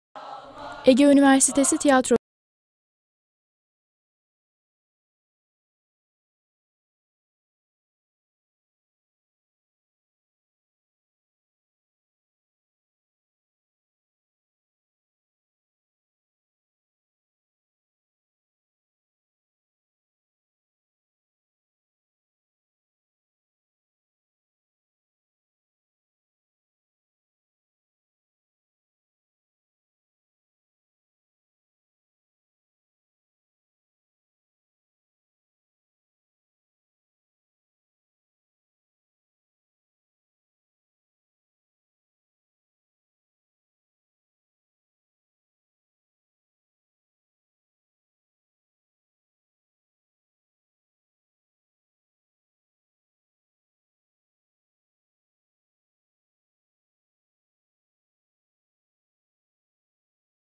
Getto Seyirciyle Buluştu
Ege Üniversitesi'nde tiyatro izleyicisine keyifli zaman geçirten Tiyatro Topluluğu, "Getto" isimli oyunla ilgi görmeyi sürdürüyor. Genç oyuncular sahneye koydukları oyunculuk performansıyla yine büyük alkış topladı.